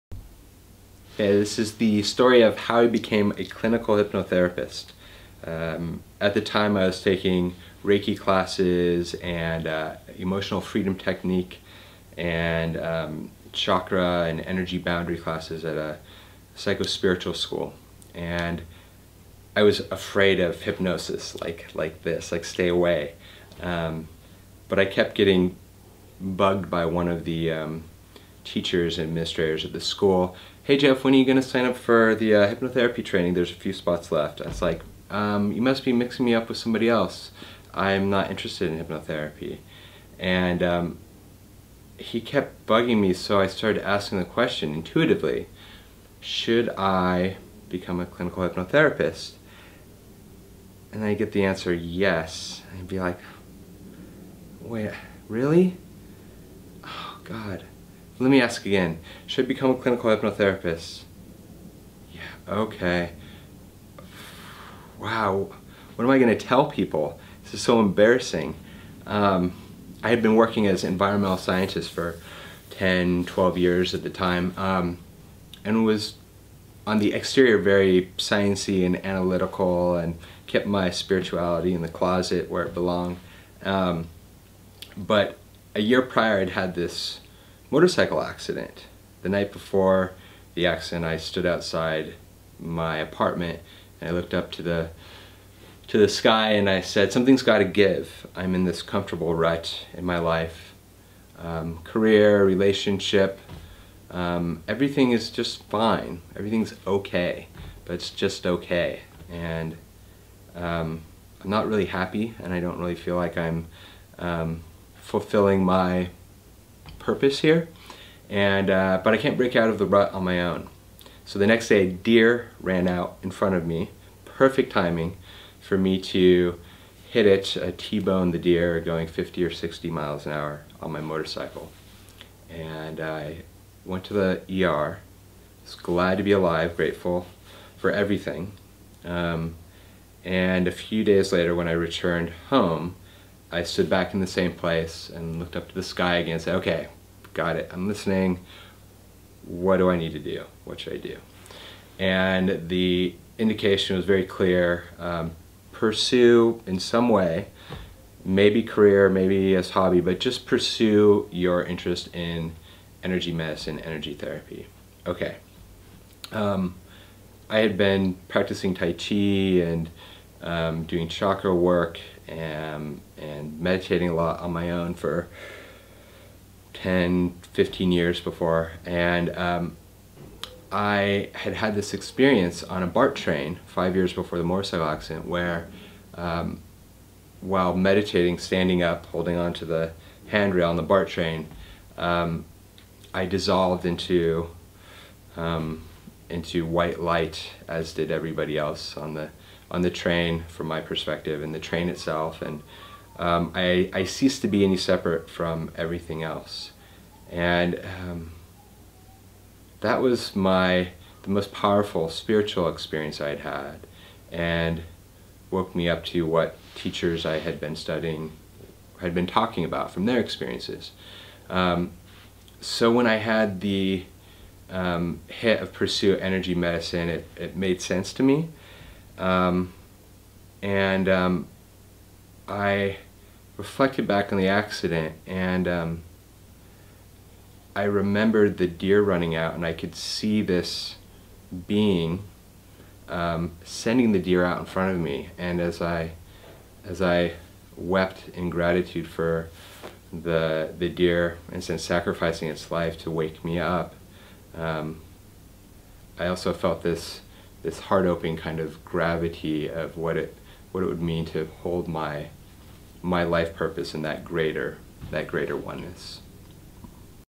Jeff Ennor, Oakland, CA
A motorcycle accident breaks Jeff out of his rut and harks him back to a moment of Oneness on a BART train.